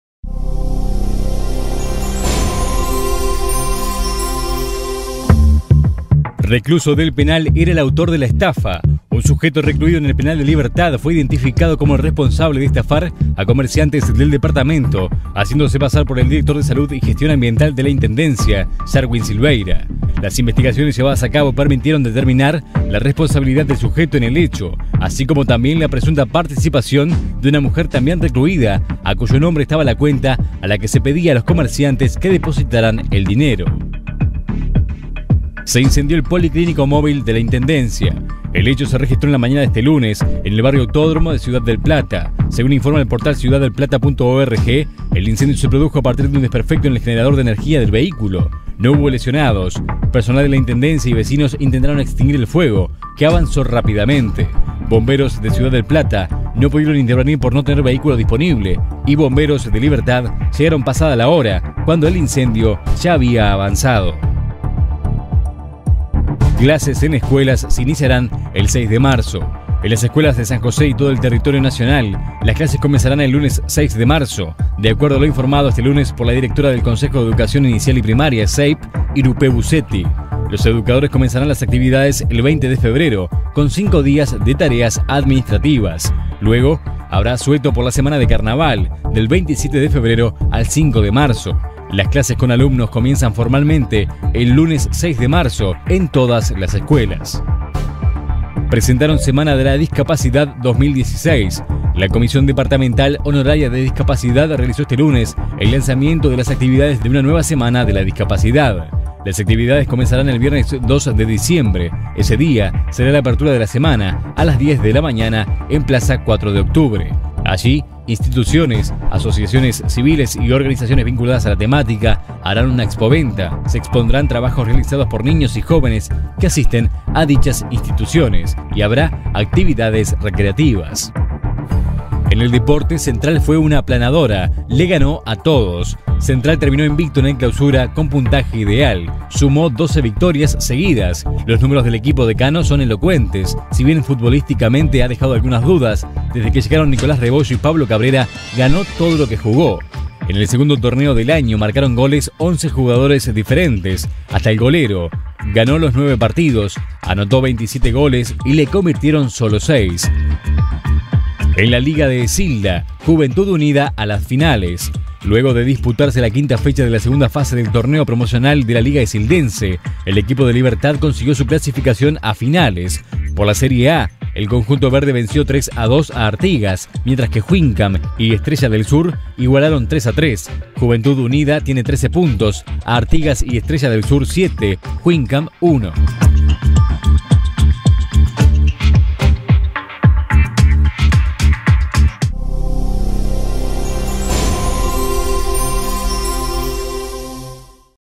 Noticias lunes 28 de noviembre de 2016
Estas son las noticias que tenés que conocer de este lunes 28 de noviembre de 2016 en el departamento de San José.